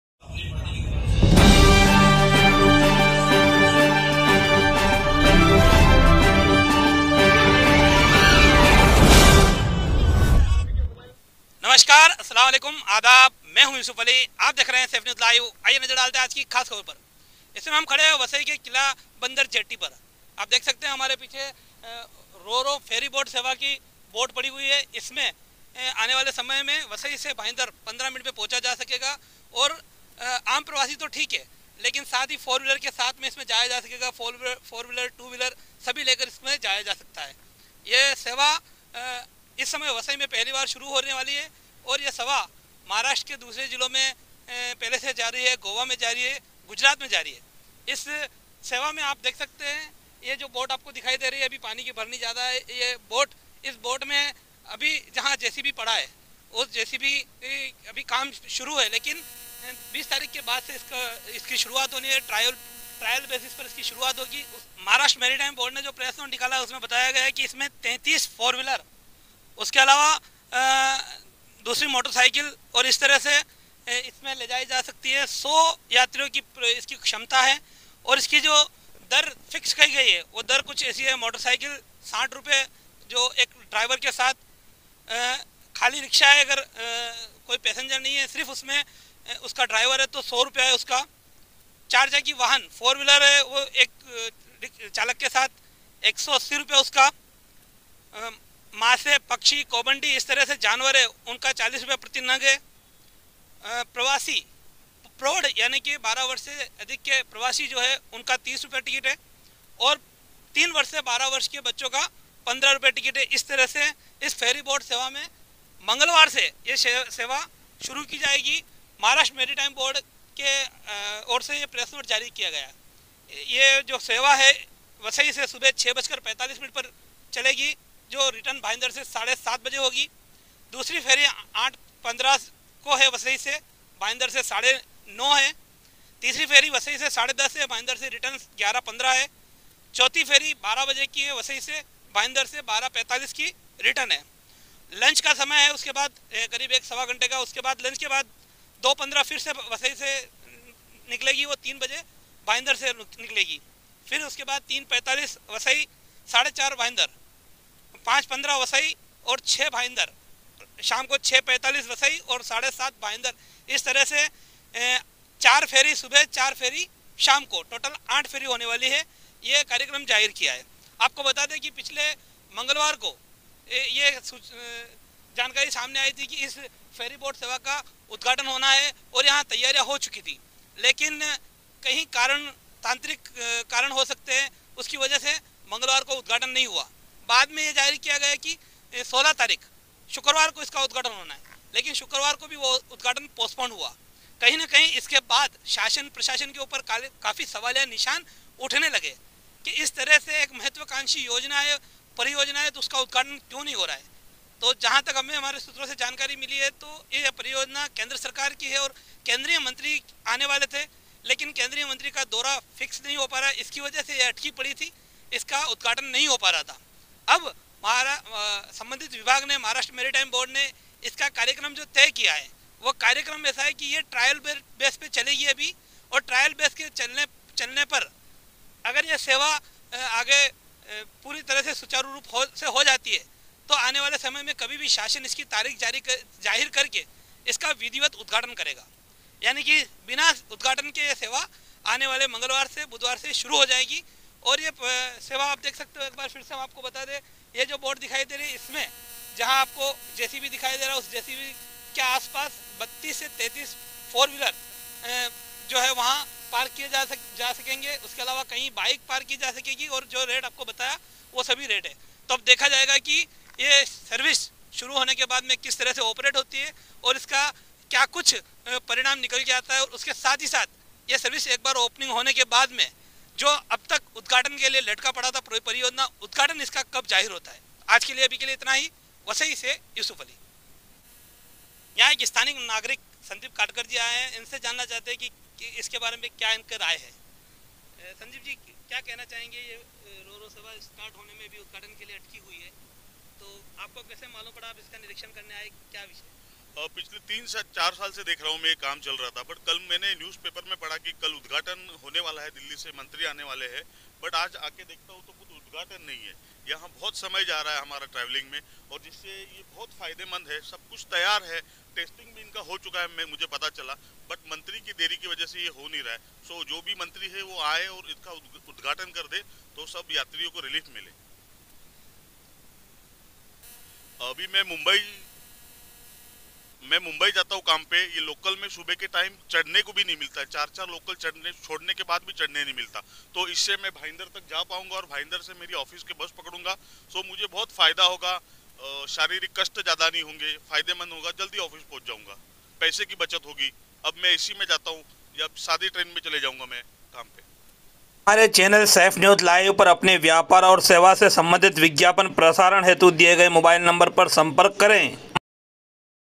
"Sailing the Seas: A Ferry Boat Adventure" Vasai to Bhayandar Ferry Boat Detail, Timming, Prices.
Vasai to Bhayandar Ferry Boat Detail, Timming, Prices. बड़ी खुशखबर वसई से 15 मिनट मे भायंदर.